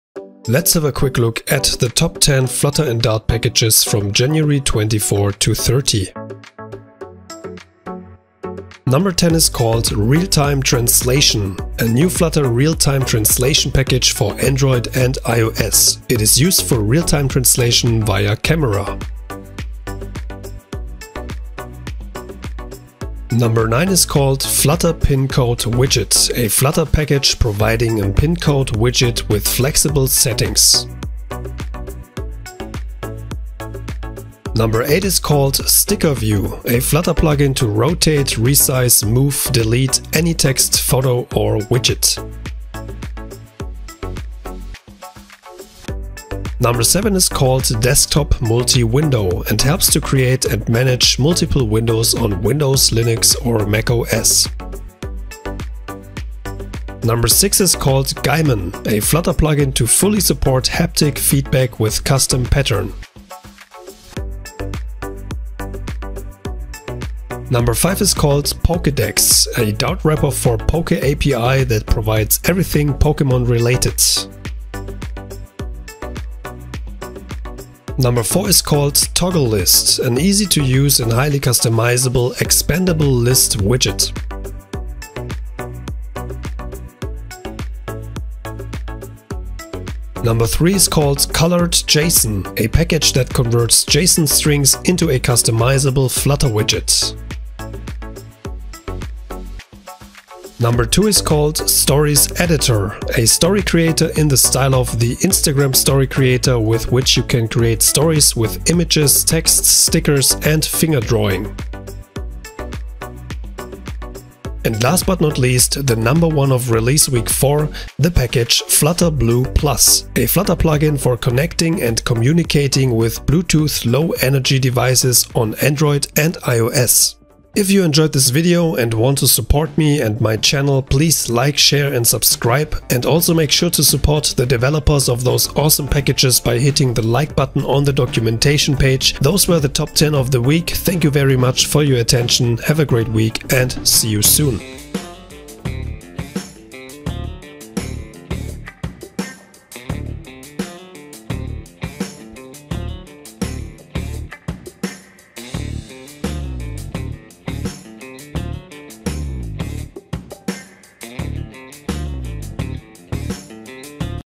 Flutter Bluetooth, Desktop Window Manager & Co. - 04 - PUB.DEV RELEASES 2022
Week 04 brings a Flutter Bluetooth package, a window manager for desktop and many more packages.

🕒 TIMESTAMPS:
00:00 - opening
00:08 - realtime_translation
00:22 - flutter_pin_code_widget
00:34 - sticker_view
00:46 - desktop_multi_window
00:56 - gaimon
01:07 - pokedex
01:17 - toggle_list
01:29 - colored_json
01:40 - stories_editor
01:54 - flutter_blue_plus
02:07 - closing